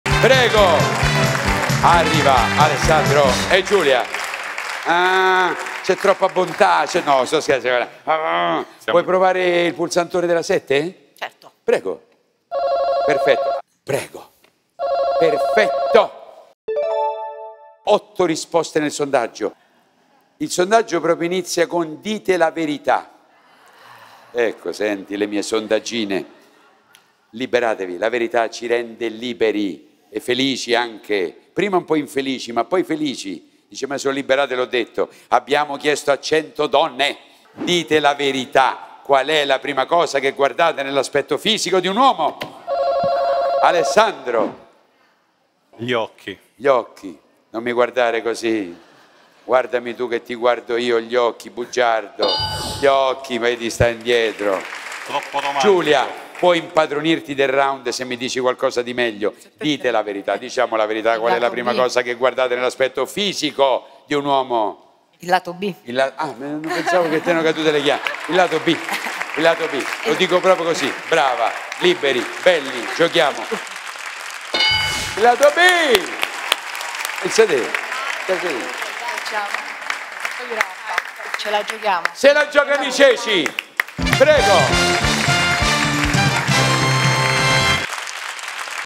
"Donne, dite la verità!"
Inizia il primo round: "Qual è la prima cosa che guardi nell'aspetto fisico di un uomo?"